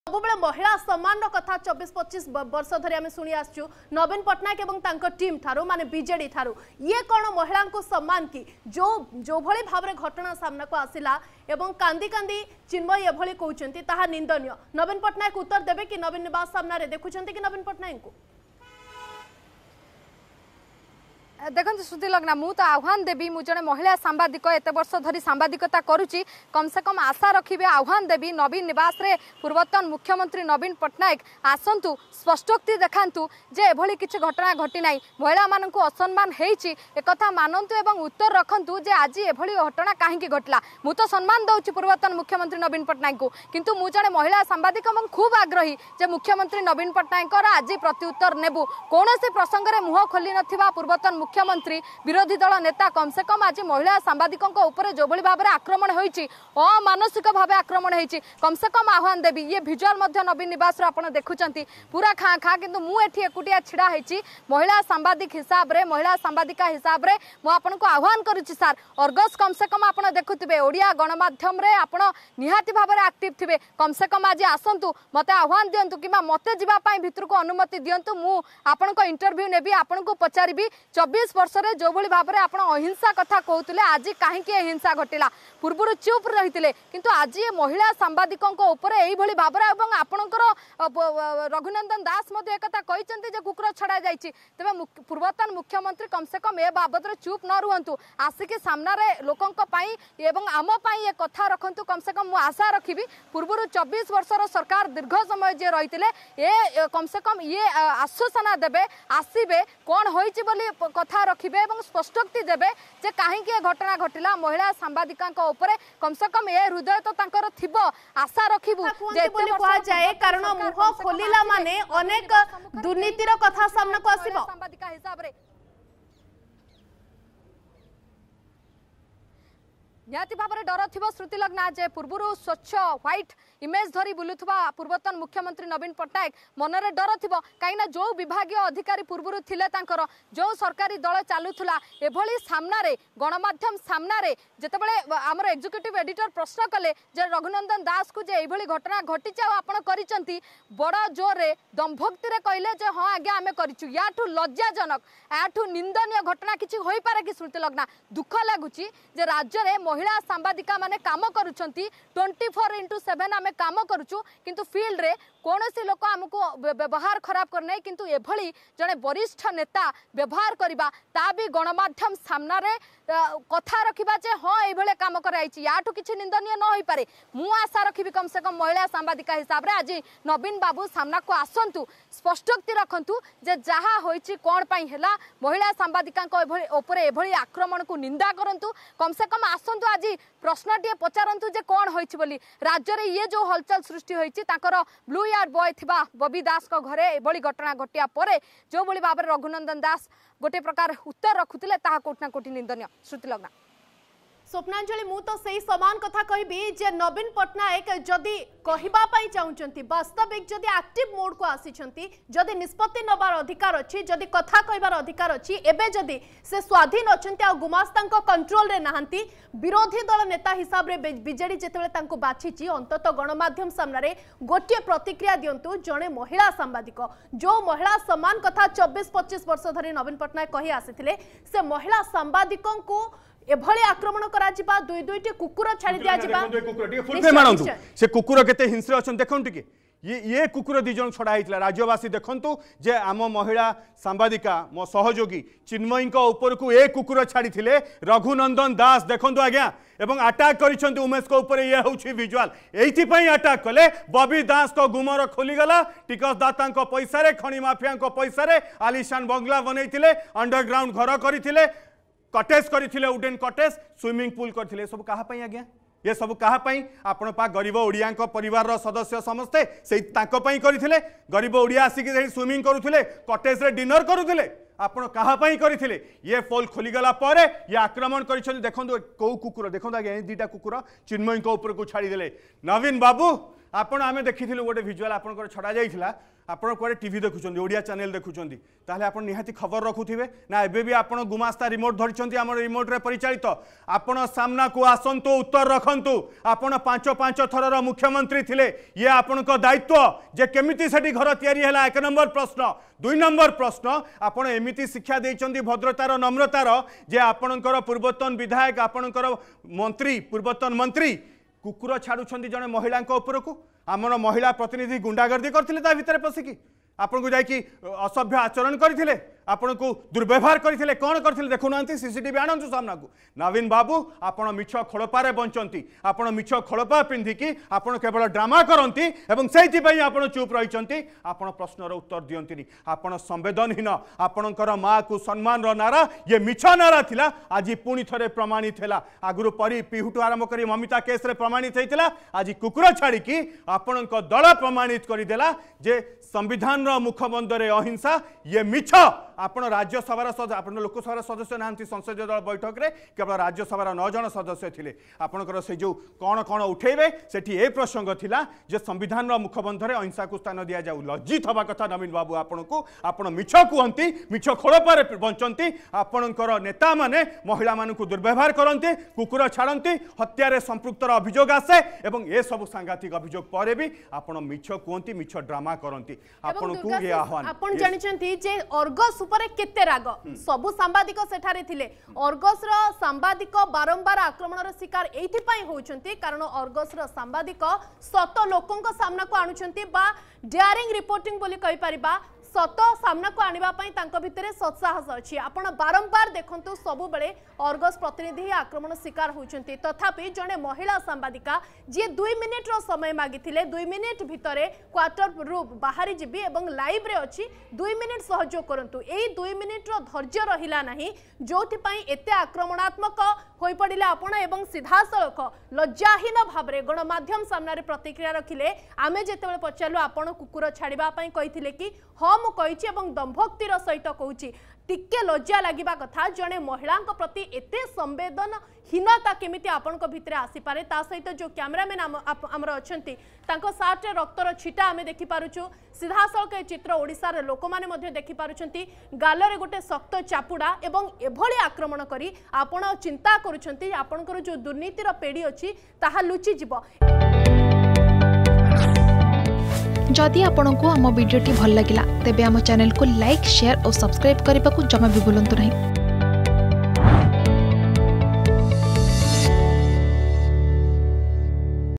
ପ୍ରତିଶୋଧ ପାଇଁ ଅର୍ଗସ୍ ମହିଳା ସାମ୍ବାଦିକା ଓ କ୍ୟାମେରାମ୍ୟାନଙ୍କ ଉପରେ କୁକୁର ଲଗାଇଦେଲେ !
ବବି ଦାସଙ୍କ ଆଲିସାନ ଅଣ୍ଡରଗ୍ରାଉଣ୍ଡ ଘରର ପୋଲ ଖାଲିବାପରେ ରାଗି ଗଲେ ପାଖରେ ରହୁଥିବା ବିଜେଡି ନେତା ରଘୁନନ୍ଦନ ଦାସ । ପ୍ରତିଶୋଧ ନେବା ପାଇଁ ଅର୍ଗସ୍ ମହିଳା ସାମ୍ବାଦିକା ଓ କ୍ୟାମେରାମ୍ୟାନଙ୍କ ଉପରକୁ କୁକୁର ଛାଡ଼ିଲେ ।